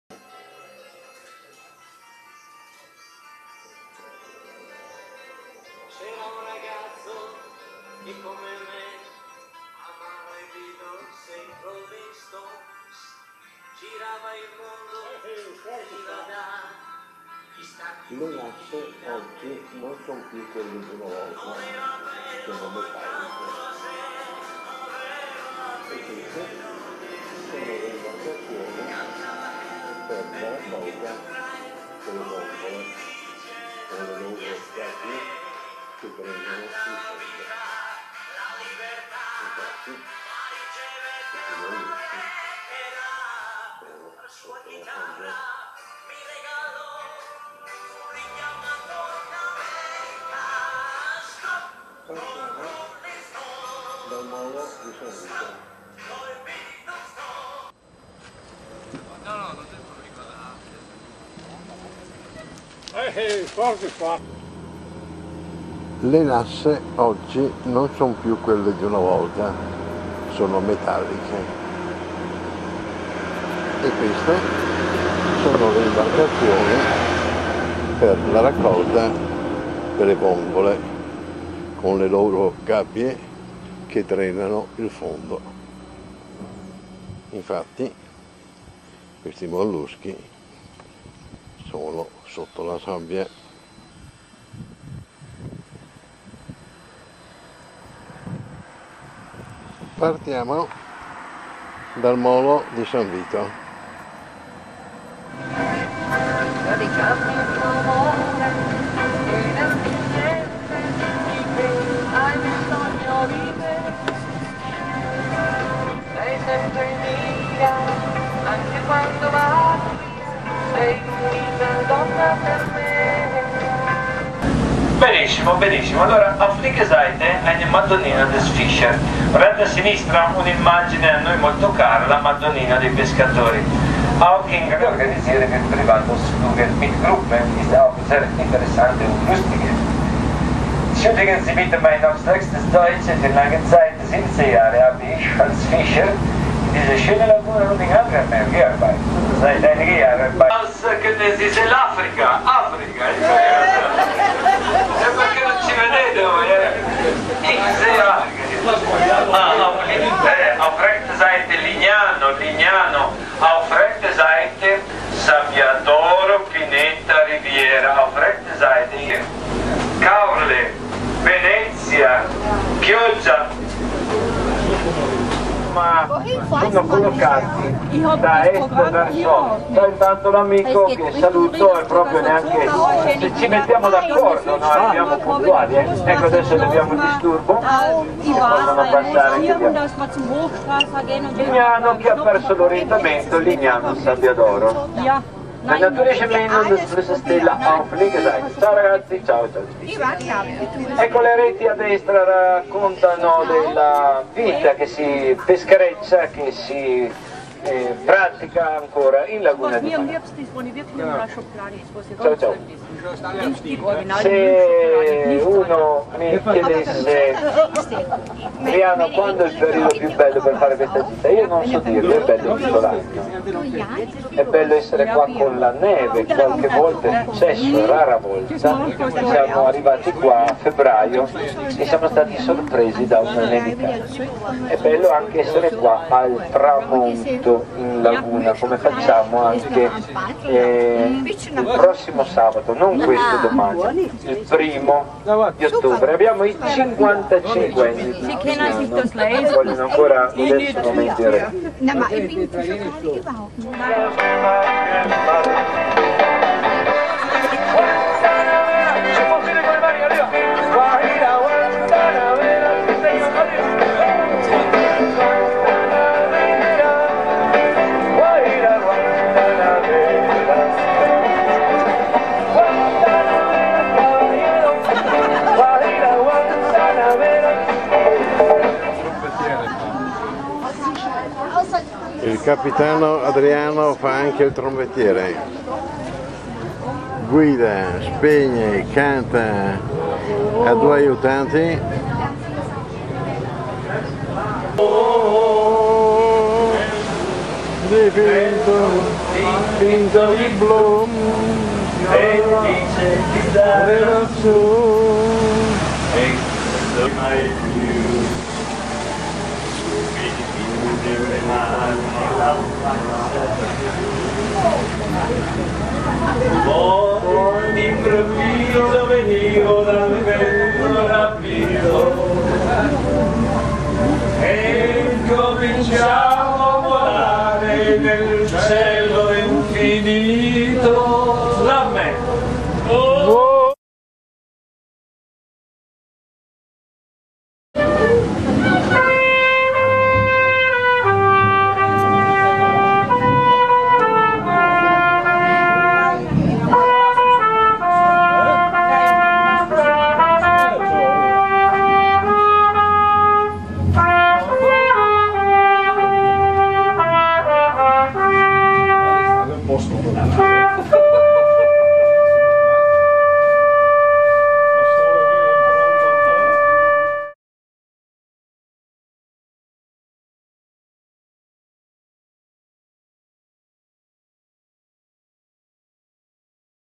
in barca con capitan geremia.mov
il 21 aprile 2012 andremo in GITA SCOLASTICA CON I 70ENNI CHE NEL 1962 HANNO SOSTENUTO LA MATURITA' AL LICEO DON BOSCO DI PORDENONE!